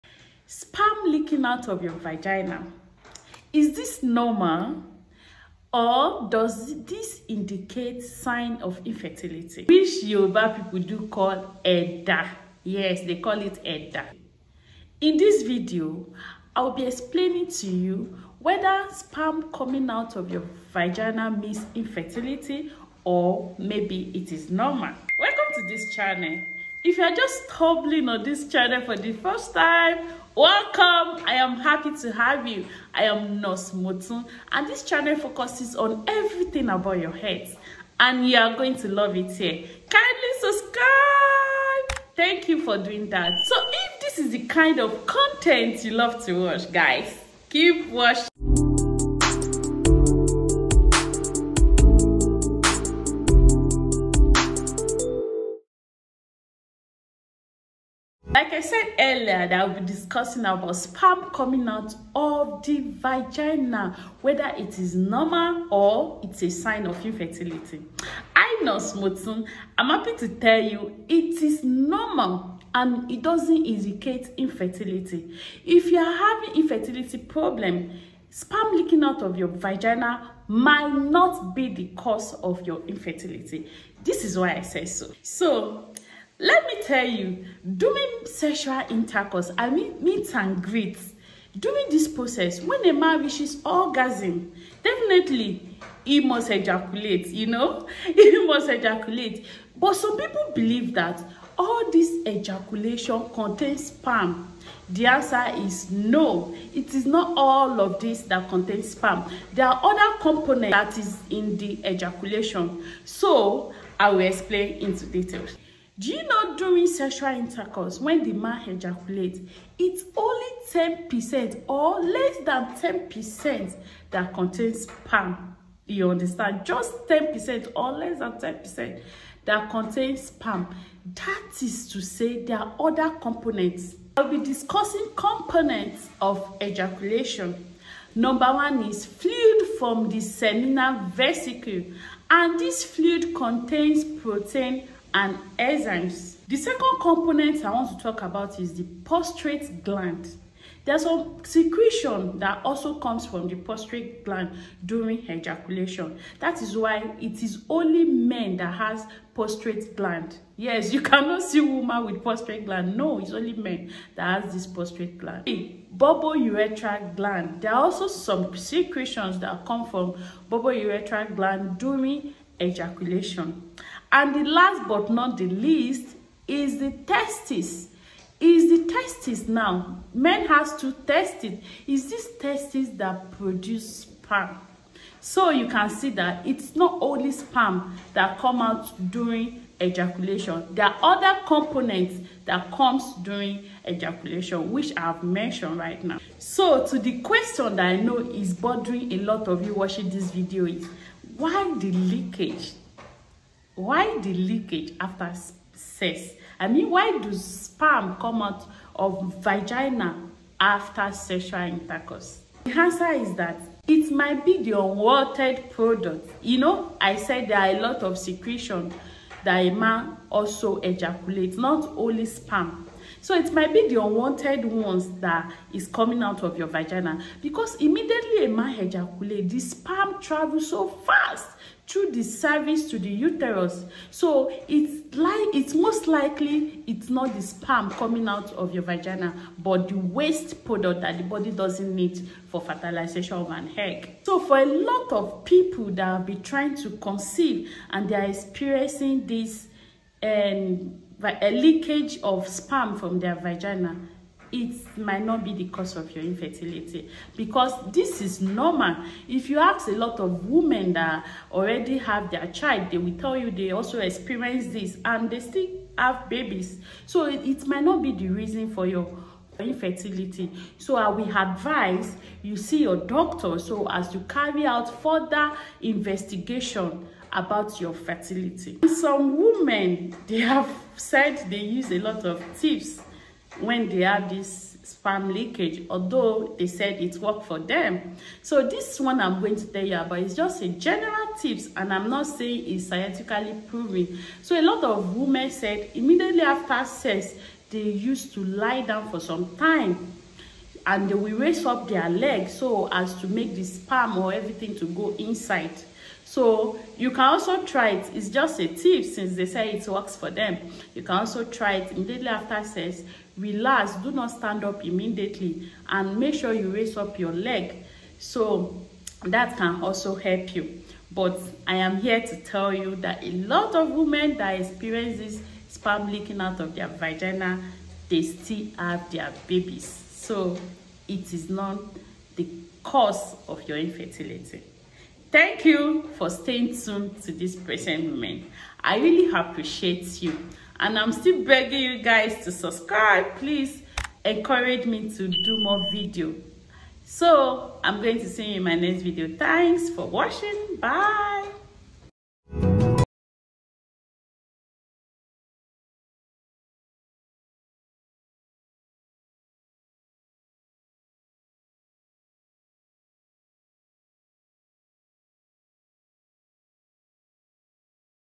SP£RM COMING OUT AFTER S**X/IS THIS NORMAL?/IS IT THE CAUSE OF INFERTILITY?
Sperm coming out of the vagina after sex. In this video, I discussed into details why sperm comes out of the vagina immediately after sex. Whether you experience this and u felt is the cause of your infertility, this video is for you and you will also gain more insights to know whether this is a normal
process or the main cause of Infertility. Join me as we explore the mechanisms behind this natural process and shed light on the factors influencing it.

⏰ TimeStamps ⏰
00:00 - Intro
01:11- Sperm Coming Out is Normal
01:43 - Ejaculation
02:16-  Component Of Ejaculation
03:24- Why The Leakage After Sex
03:51- Sperm Travel So Fast
04:16 - It Might Not Be The Cause Of Infertility
04:40 - General Tips
05:37 - Appreciating The Content